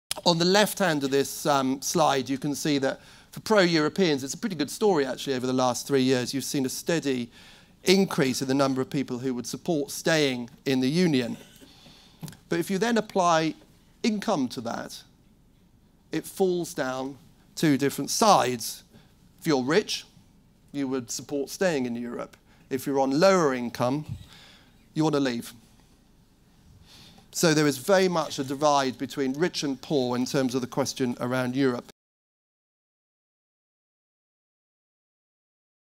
Edelman Trust Barometer 2016: Trust gap evident within policy issues such as
The Edelman Trust Barometer survey is the firm's annual trust and credibility survey. It measures trust across a number of institutions, sectors and geographies. The 2016 Edelman Trust Barometer surveyed more than 33,000 respondents across 28 countries.

Here Ed Wiliams, CEO, Edelman UK & Ireland, explores gap between rich and poor in attitudes towards the EU.